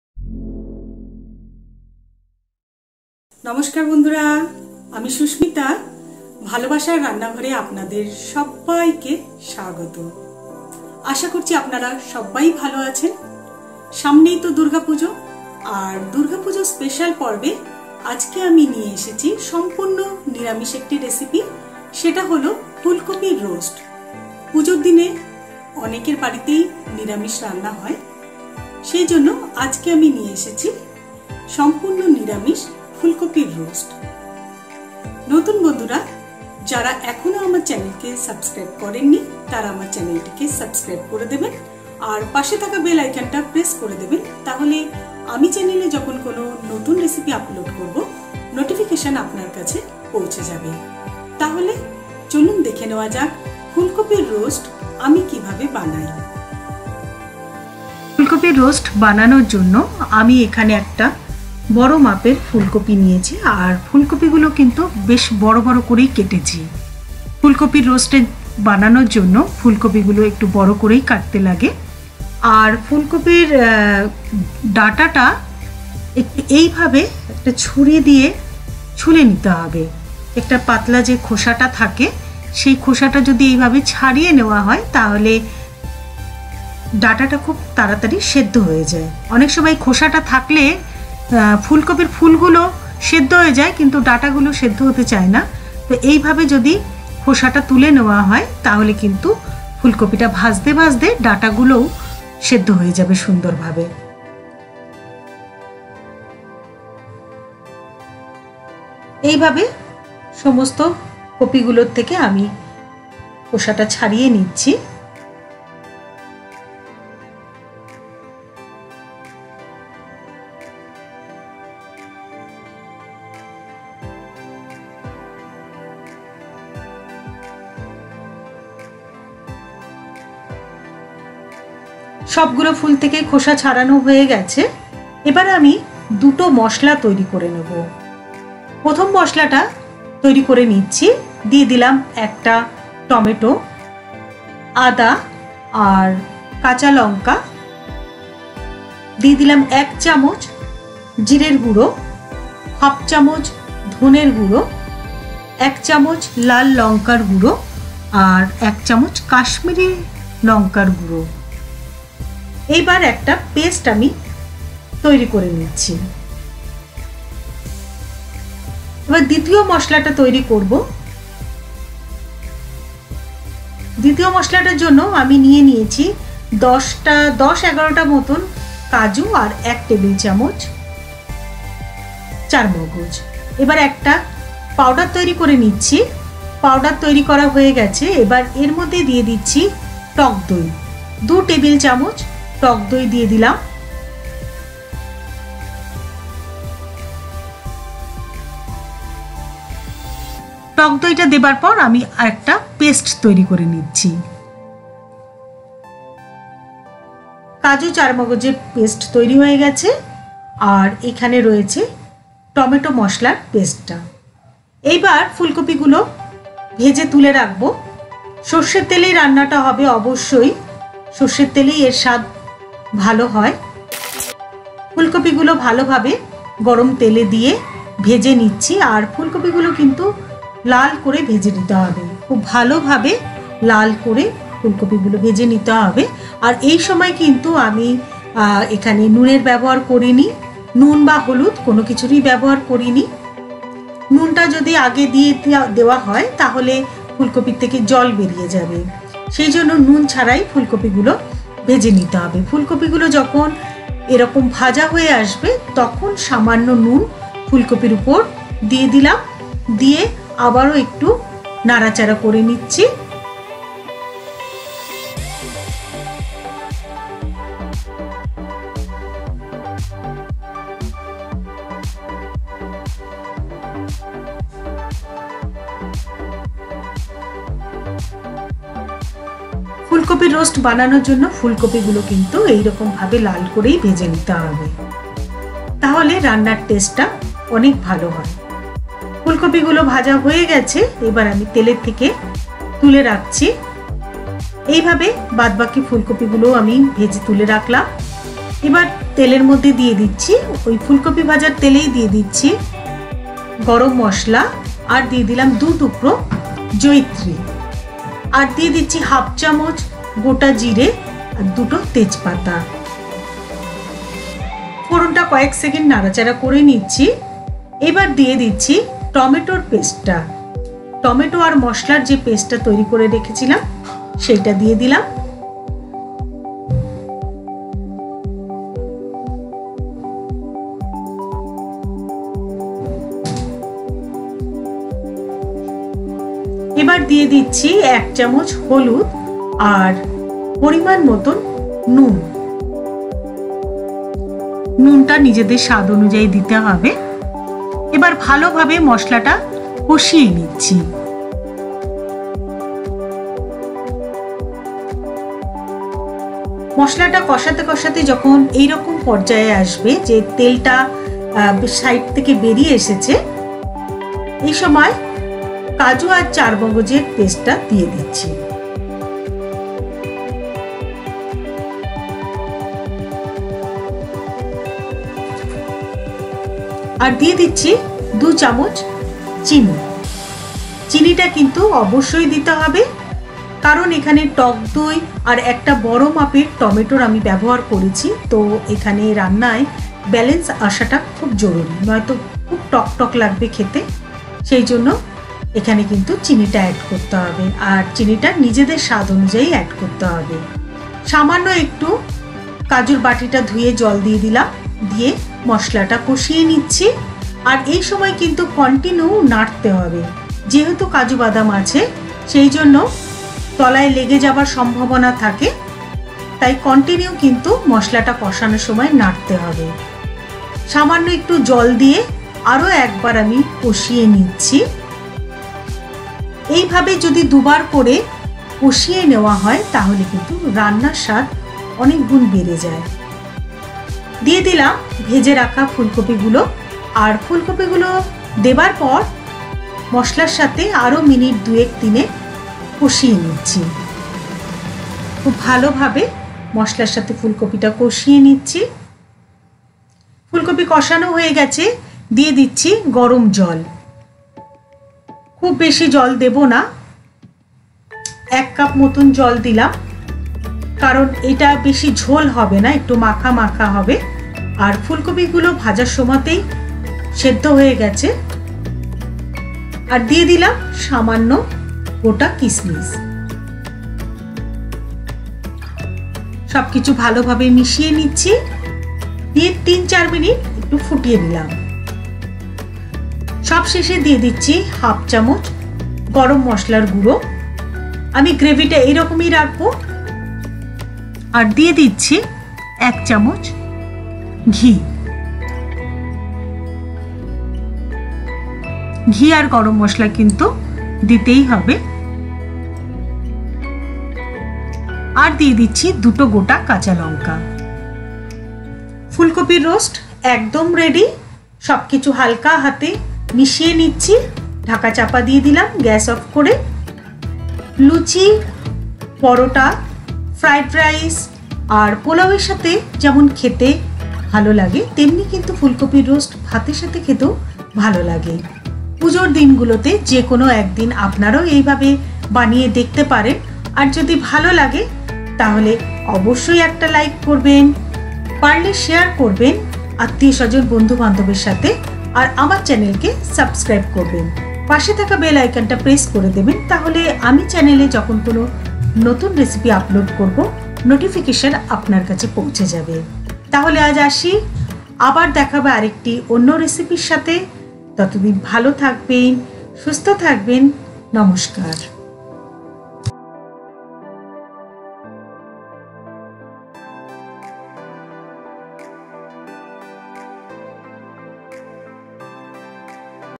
Fulkopir roast | দুর্গাপূজা স্পেশাল। ফুলকপির রোস্ট রাজকীয় স্বাদে নিরামিষ ভাবে বানিয়ে ফেলুন
INGREDIENTS :-

Fulkopi - 1big sized
Mustard oil - as you required
Salt - according to test
Turmeric powder - 1tsp
Sugar - according to test
Garam masala whole
Javitri
Raisins
Garam masala powder - 1/2tsp
Ghee - 1tsp

INGREDIENTS FOR MASALA  - 1

Tomato - 1big sized
Ginger - 1"

INGREDIENTS FOR MASALA  - 2

Cashew nut - 10 - 11
Char mogoj - 1tbsp
Curd- 2tbsp